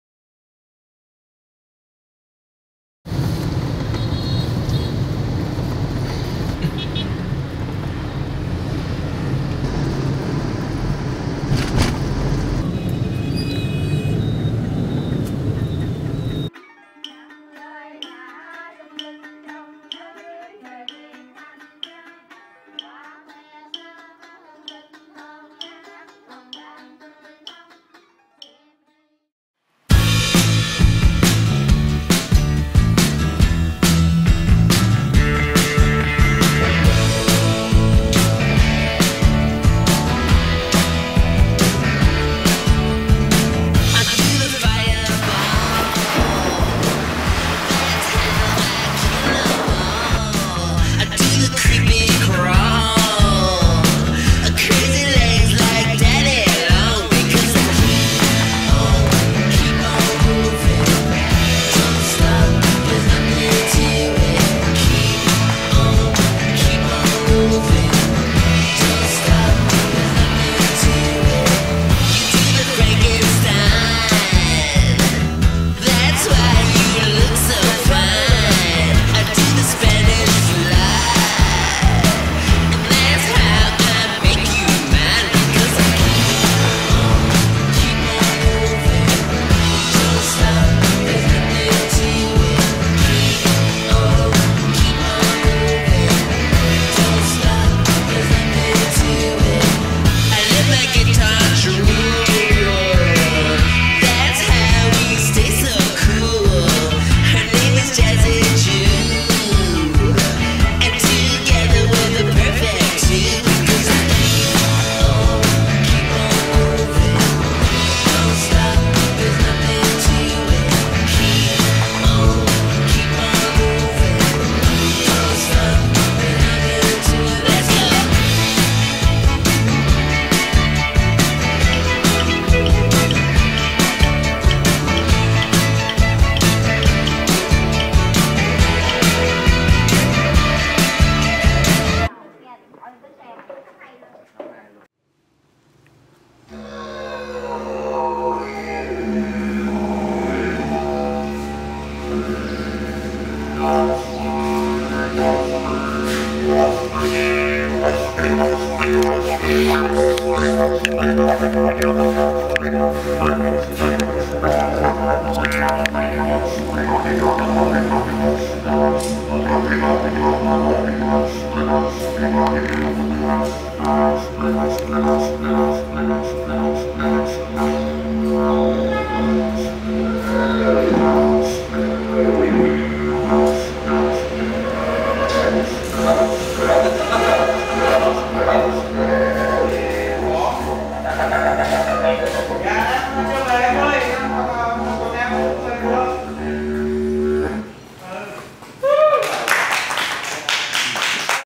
Biochar Production at EcoFarm with a BiGchar 2200
Biochar production with BiGchar technology at EcoFarm in Vietnam.  This video briefly brings you inside the technology while it is firing.  Corn and rice residues become fuel for drying the grains, resulting in biochar that is blended with organic fertilizers and spread back into the fields.